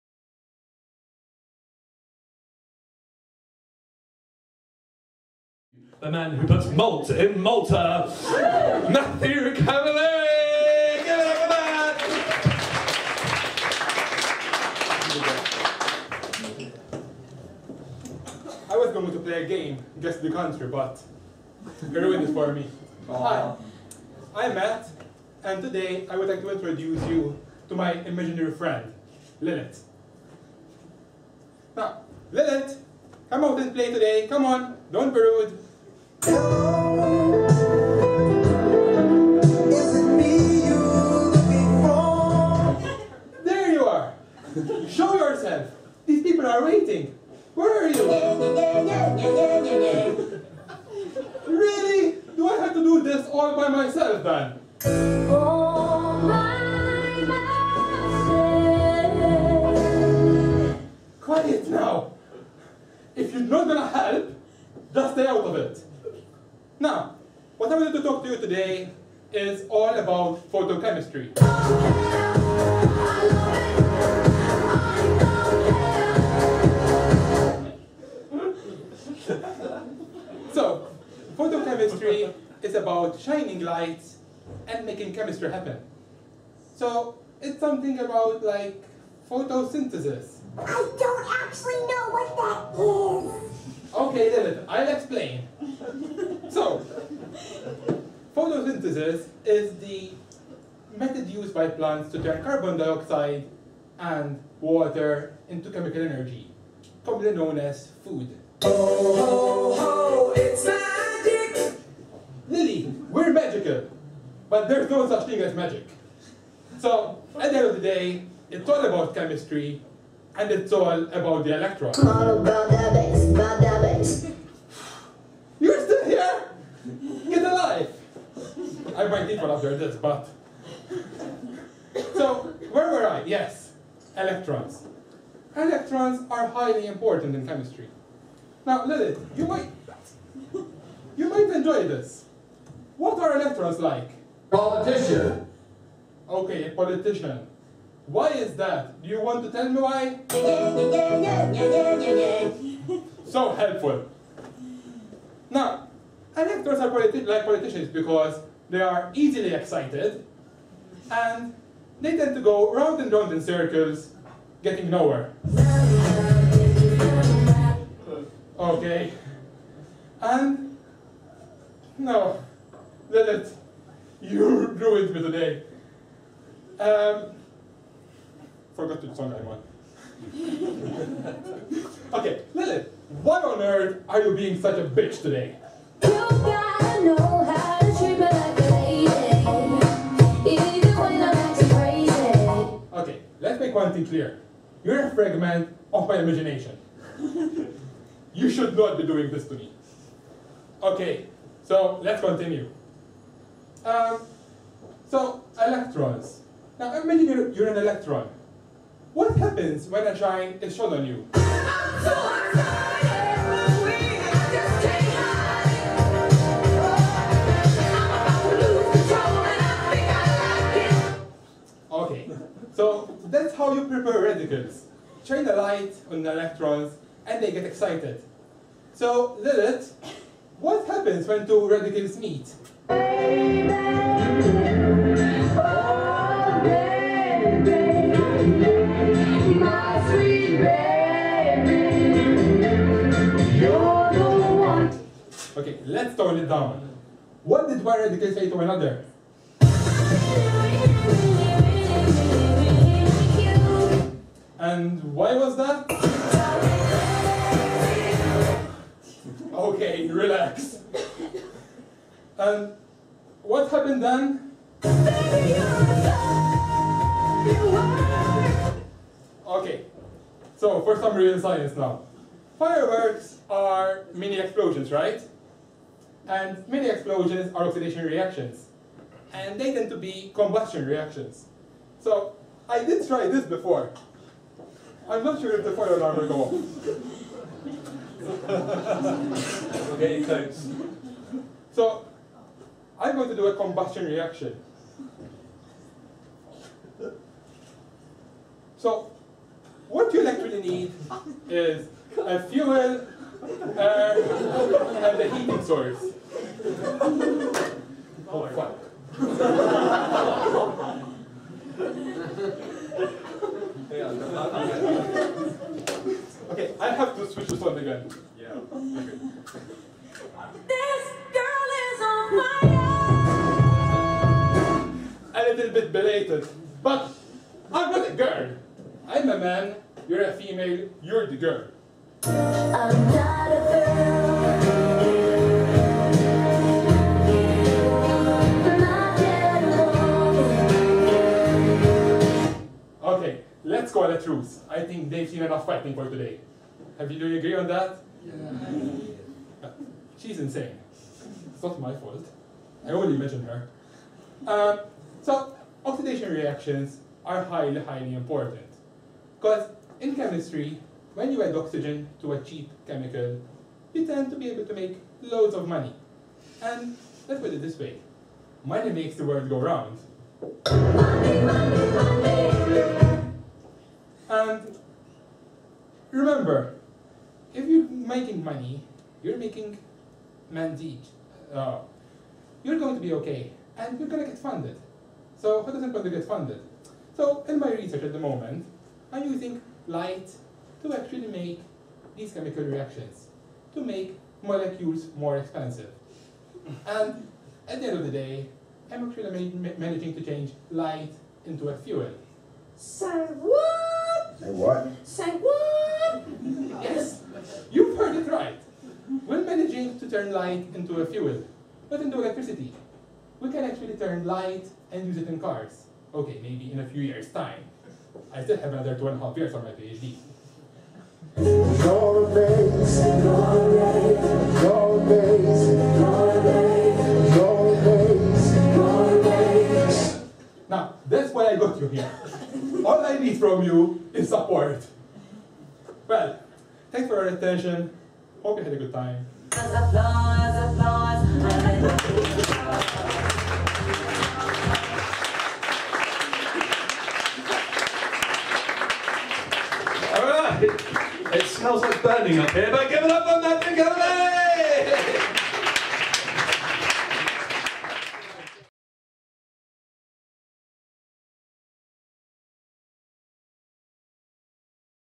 Matthew Camilleri Science Stand up
Science stand up at its best; a scientist talking to his imaginary friend. What's more, I hear you say? He also tries to set himself on fire while trying to explain what photochemistry is.